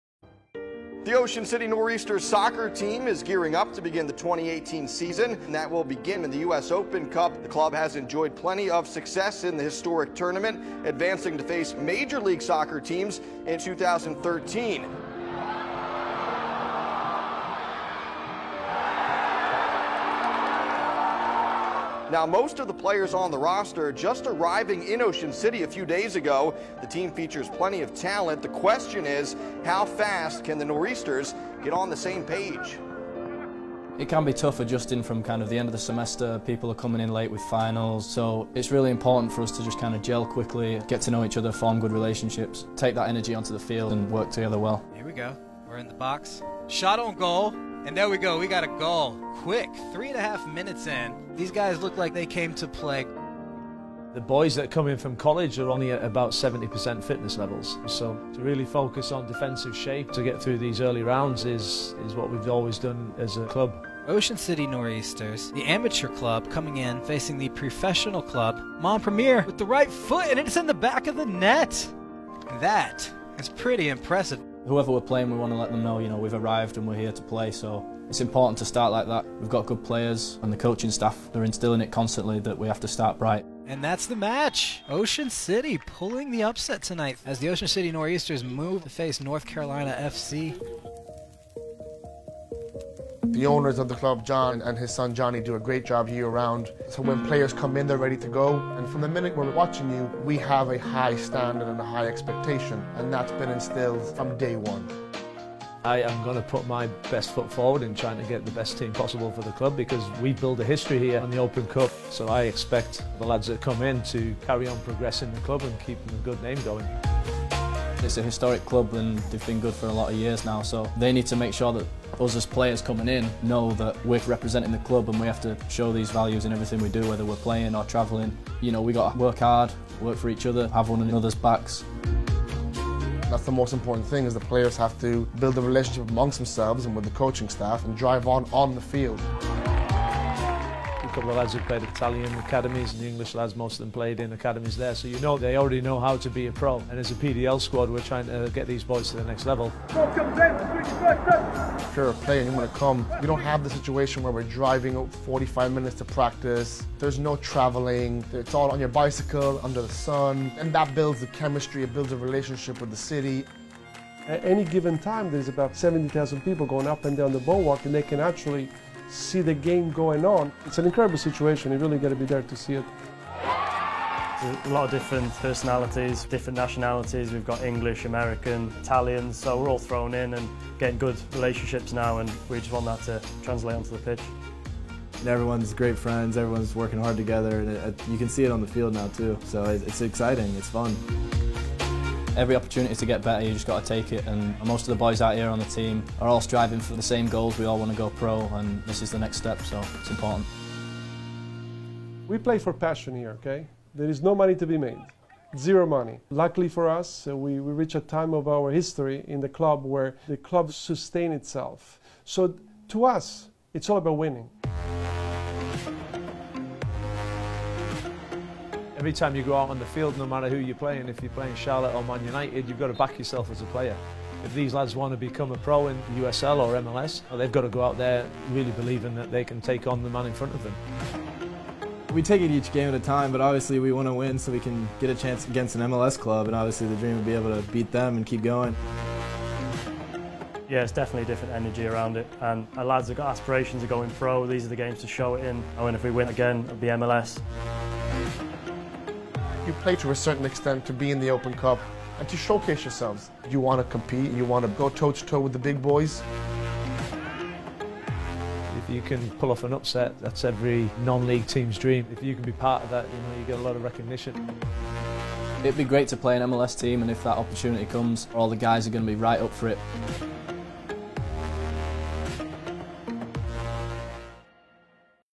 "It's all about winning" - Ocean City Nor'easters feature story from 2018
Ahead of their 2018 US Open Cup match against North Carolina FC, the United States Soccer Federation produced a feature story about the Ocean City Nor'easters, the club's rich history and its commitment to winning while representing the city of Ocean City, N.J., America's Greatest Family Resort.